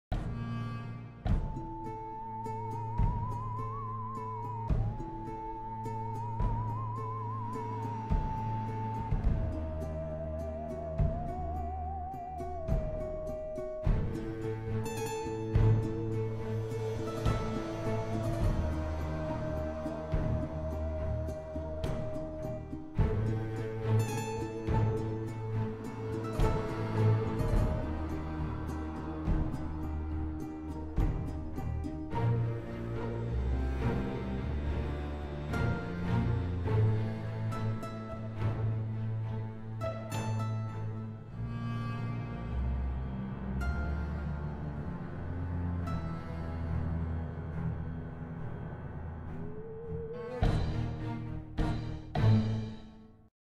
Glacier Collapse in İndia -Hindistanda Buzul Felaketi
India's glacier collapse: At least 19 dead, more than 200 missing.Feb. 8, 2021